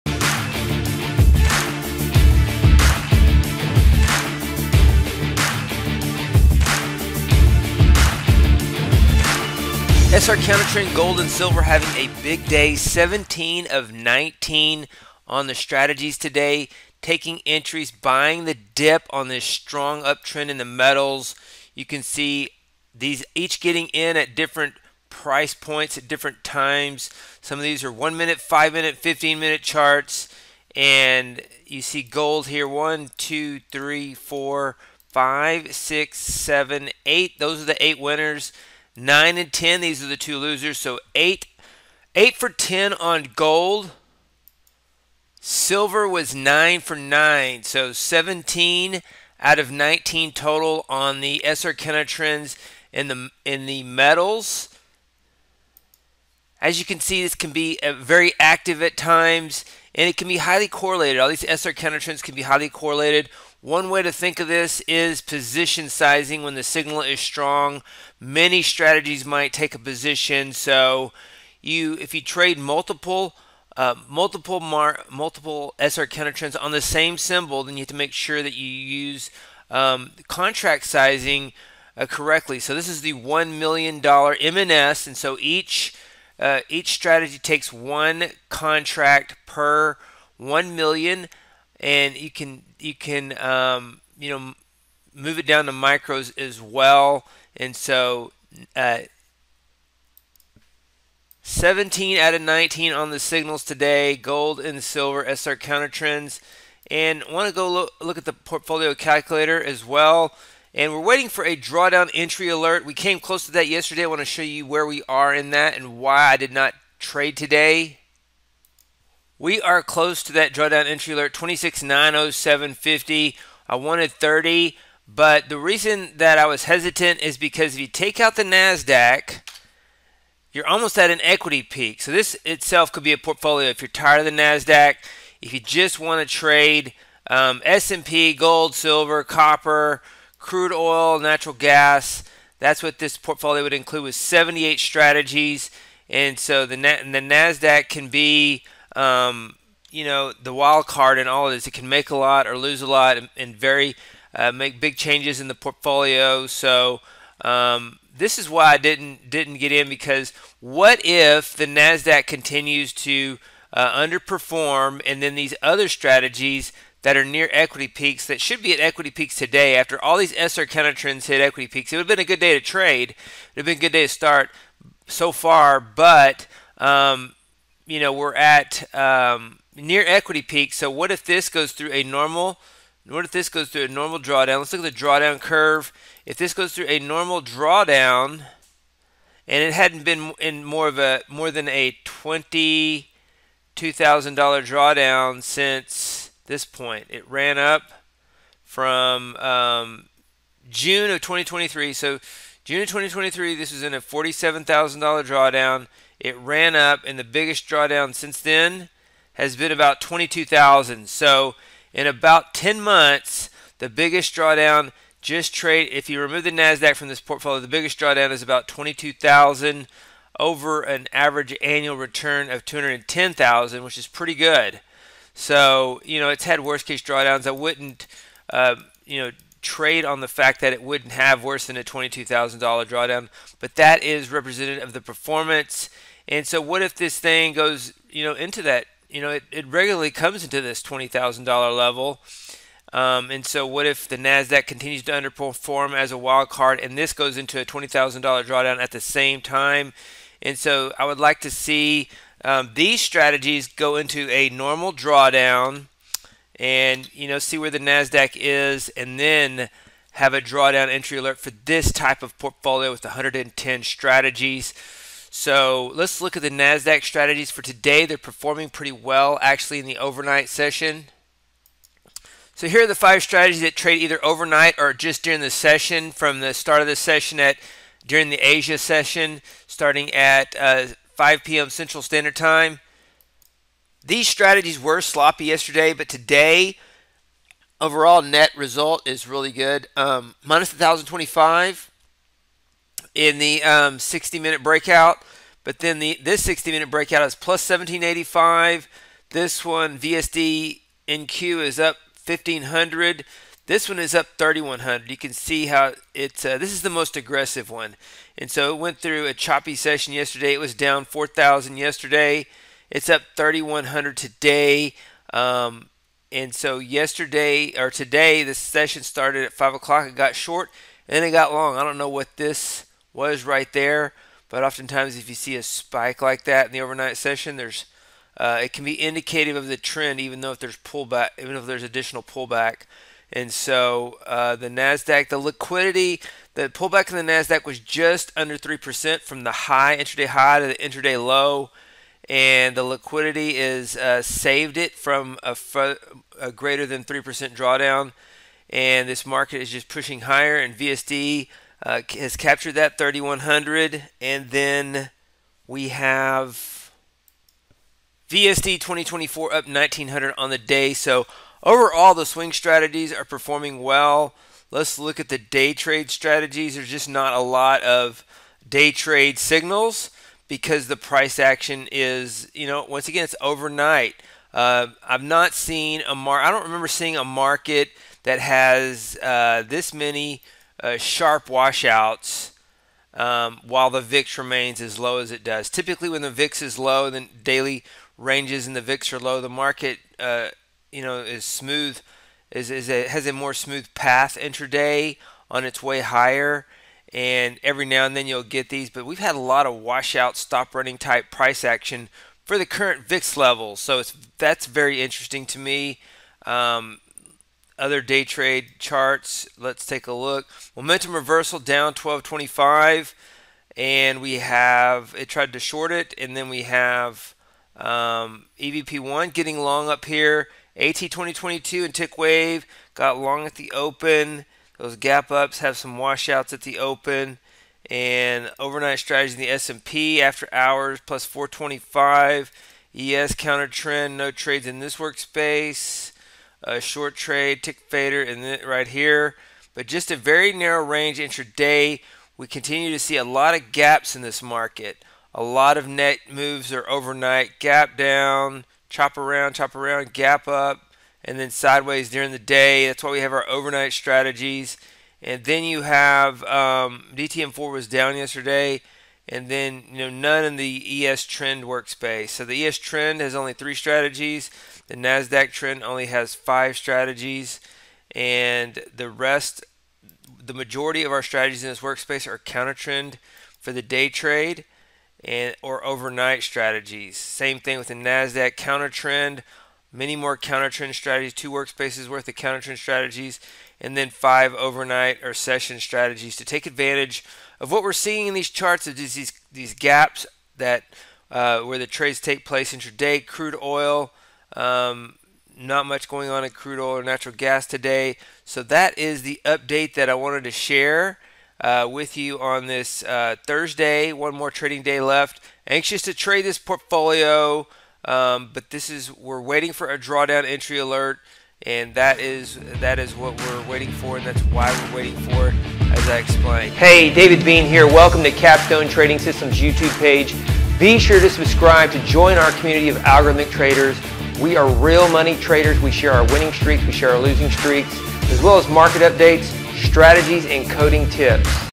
High Accuracy Trades in Metal Futures 17 Winners on 19 Trades
The SR CounterTrend Gold and Silver strategies are active today calling 17 of 19 trades with 89% winners. These strategies are correlated. One way to understand using correlated and similar strategies on the same market or market sector is to think of it in terms of position sizing relative to signal strength.

We are waiting for a drawdown entry alert to go live. We have not traded live this month. We were close to a drawdown entry alert yesterday but most of the drawdown was in the Nasdaq. The Nasdaq has been and still is a wildcard lately. The combination of other markets are hitting equity peaks today. I don't want to be in a scenario where the Nasdaq strategies go into a worse case drawdown while the other strategies go into a normal drawdown. The other strategies are due for a quick cyclical 20K drawdown.

We discuss how this would be a good portfolio without the Nasdaq strategies.

The Nasdaq overnight strategies are doing well capturing the move on this gap up today. The trend day trade environment in the stock indexes and especially the Nasdaq has been challenging but the overnight session has some nice opportunities to be a part of the moves that generate gap ups or gap downs.

The Nasdaq almost had a 3% pullback from intra-day highs on March 21 to intra-day low on April 2. It was saved by the bullish liquidity driving the stock indexes higher. We continue to anticipate higher prices as we discussed last year based on the biggest fundamental - liquidity plowing into assets regardless of any other fundamentals. We continue to see dip buying. The more bearish the headlines, the better the dip buying opportunity as shallow dip buying continues. We haven't seen a "normal" pullback since October.

Open Code Trading System Courses:
Seven Trading Systems for the S&P Futures